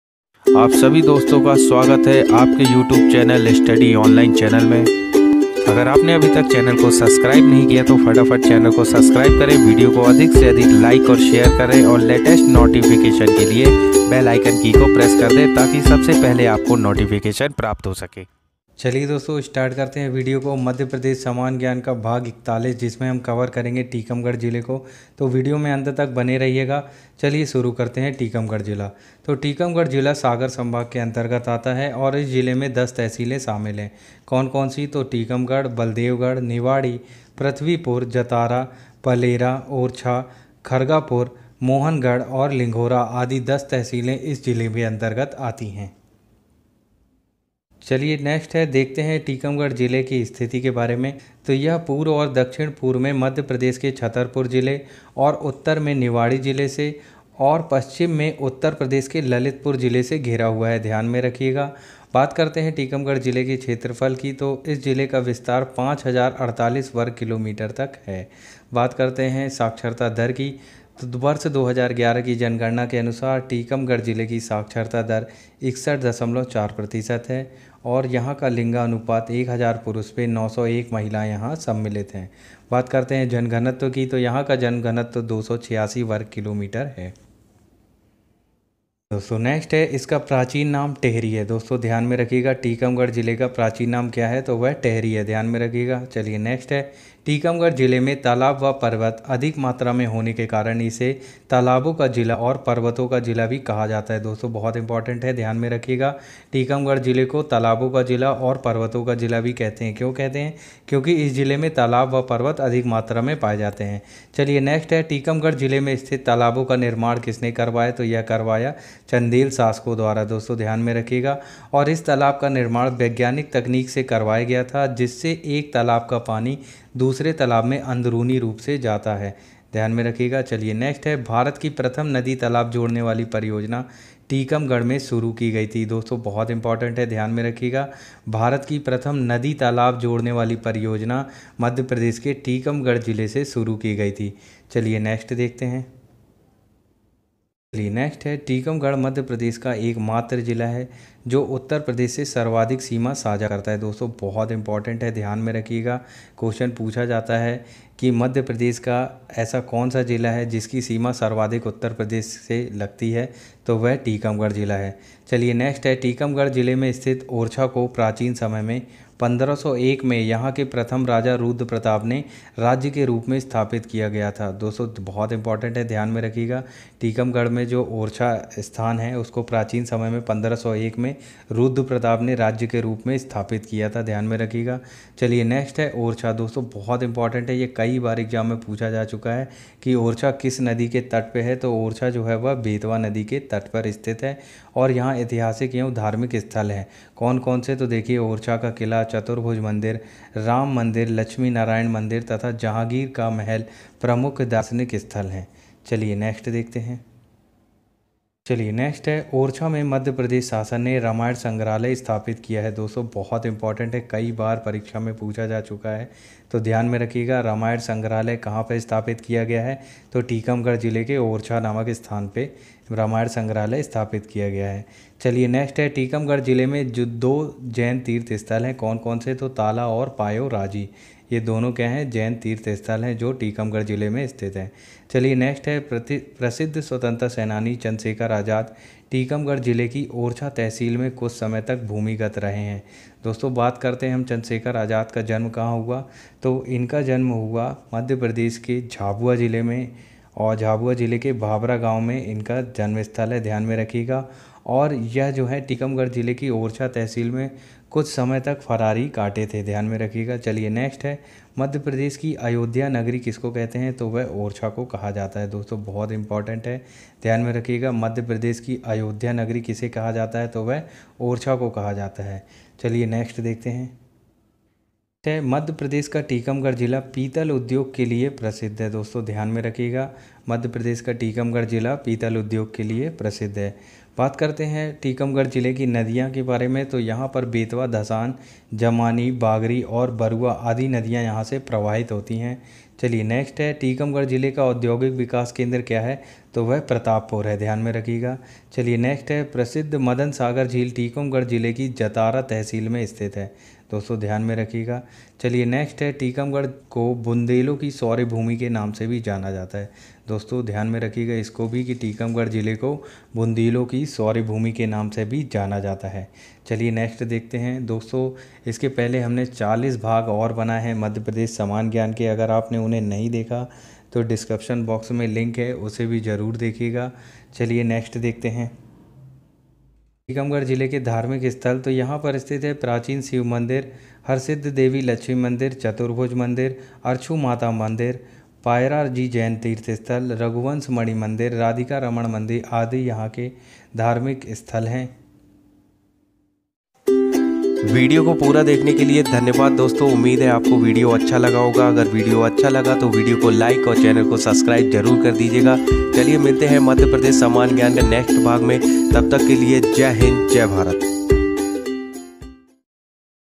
टीकमगढ़ जिला /mp police/mppsc/mp s.i/mp forest/study online / मप्र की सभी प्रतियोगी परीक्षा हेतु!
Study online
दोस्तों इस विडियो मे सागर संभाग के अंतर्गत आने वाले टीकमगढ़ जिले के महत्वपूर्ण तथ्य को कवर किया है,
इस विडिओ मे टीकमगढ़ जिले का पुराना नाम, उसके अंर्तगत परियोजना, नदी, पर्यटन स्थल, और संग्रहालय को बताया है  दोस्तों ये विडिओ मध्यप्रदेश के किसी भी आने वाली परीक्षा - जैसे mp police, mp s.i, mp patwari, mppscl
study online